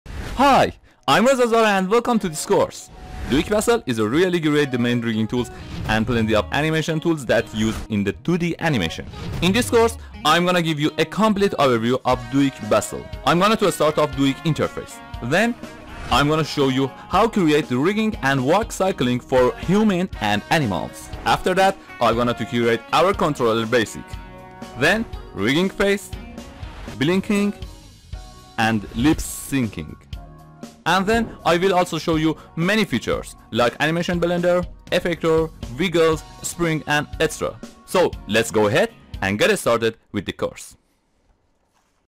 Motion Graphics with Duik Bassel 2019 in After Effects Trailer | Available on Udemy & Skillshare
Hi there,
My new class is about Duik Bassel 16 in After Effects CC 2018 is published.

If you see this course you will learn:
Character rigging for human and animals
Walk cycling
Face Rigging
Puppet pin rig and walk cycle
The best tips and Trick for Animation

Let's get start it!